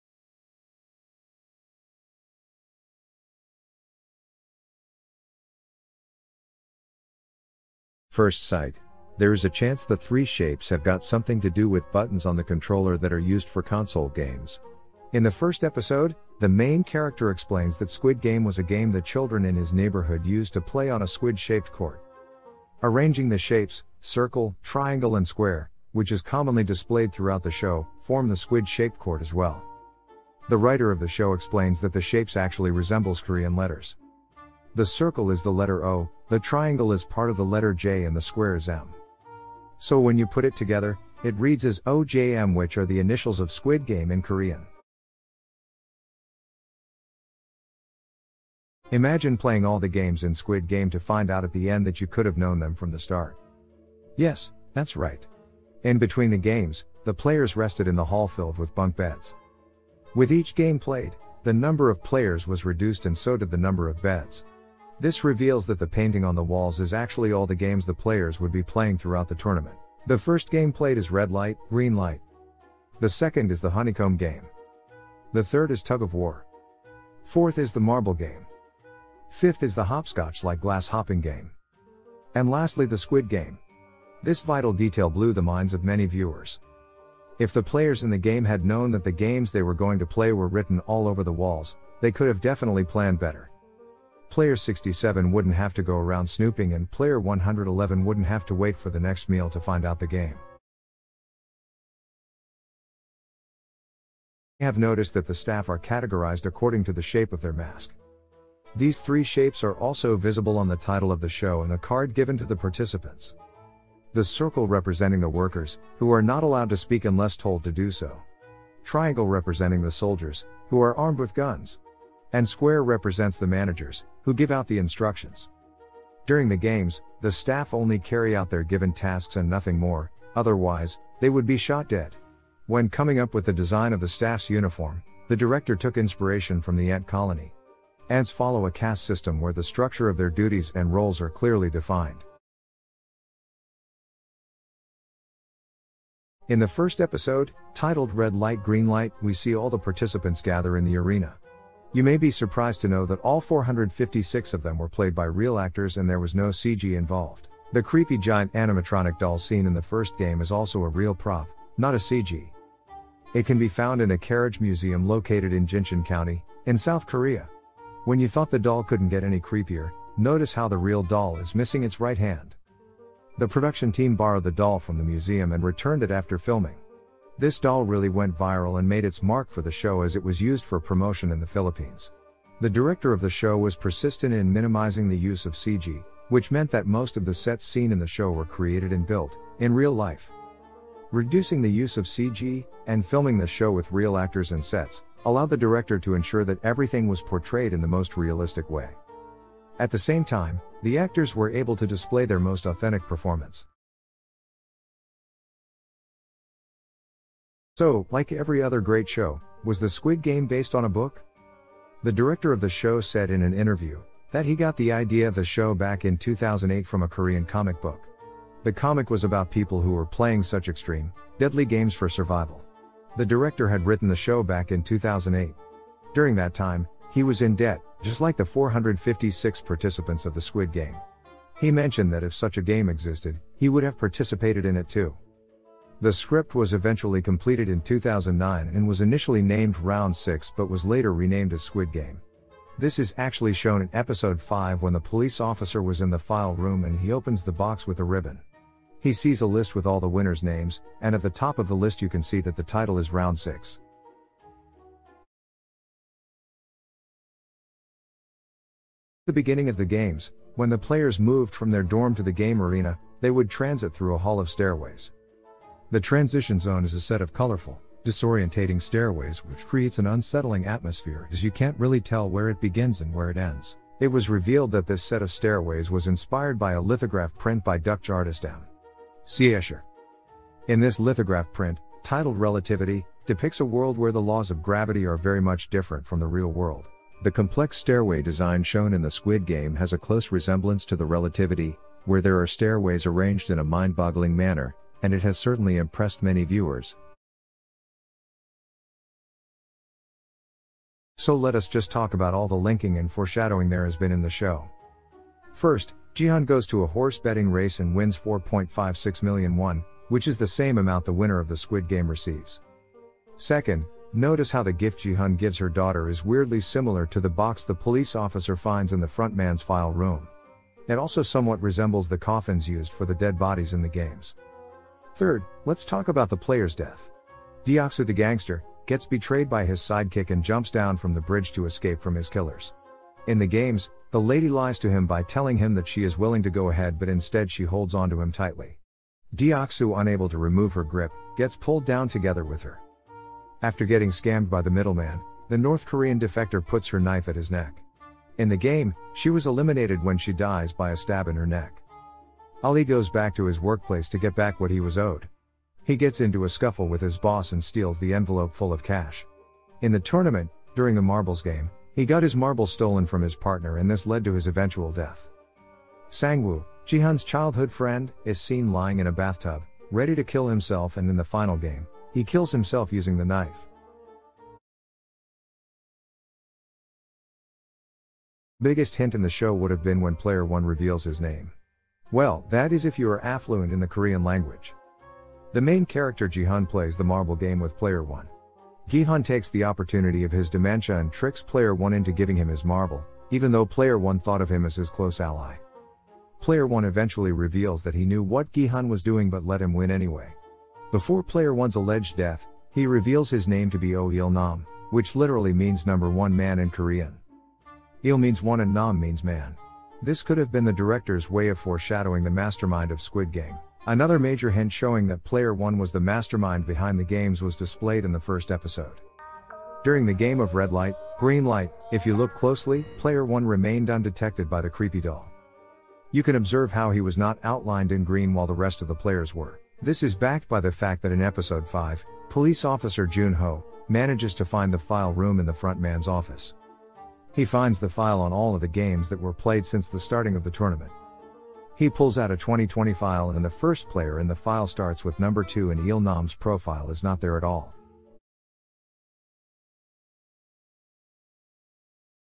SQUID GAME! DID YOU MISS THESE 8 THINGS? MUST WATCH
SQUID GAME [SPOILER] | Details and Clues you may have missed. The latest Netflix hit show Squid Game.